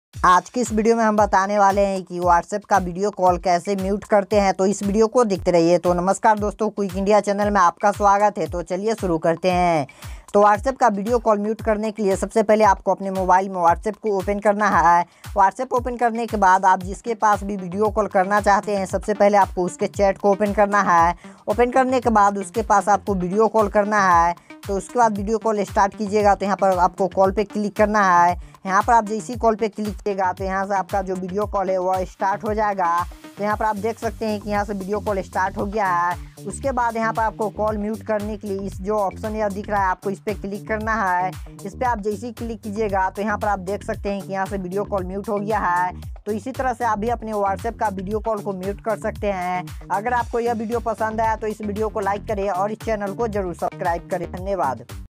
Whatsapp Video Call Mute Kaise Kare !! How To Mute Whatsapp Video Calling
Aaj ke is video me ham batane vale hai ki whatsapp ke video call ko mute or band kaise karte hain

➡ Queries Solved

1.

About :- प्रिय मित्र, इस चैनल पर कंप्यूटर और मोबाइल से संबंधित वीडियो रोज अपलोड किए जाते हैं